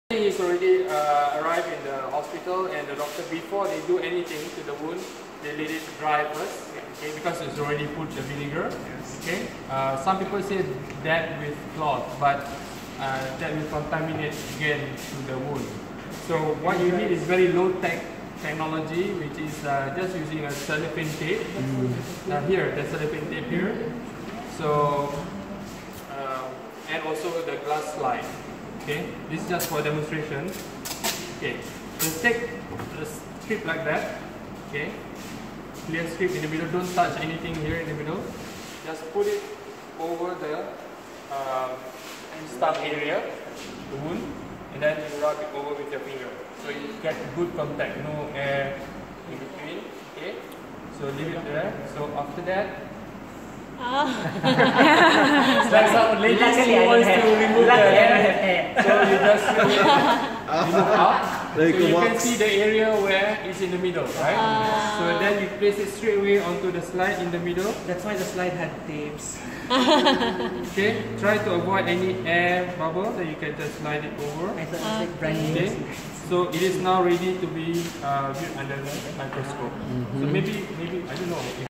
Sampling nematocysts from jellyfish sting wound
IOC WESTPAC Harmful Jellyfish Project 2019

For the examination under microscope:
Step 1. Use 10x magnification to screen for nematocyst.
Step 2. Once viewed, switch lens to 40x magnification
Step 3. Take a picture with mobile phone camera through the ocular lens.

Note: to enhance the nematocyst visualisation, adjust the mobile camera lens to 2.5x magnification before taking the photo of the of the sample.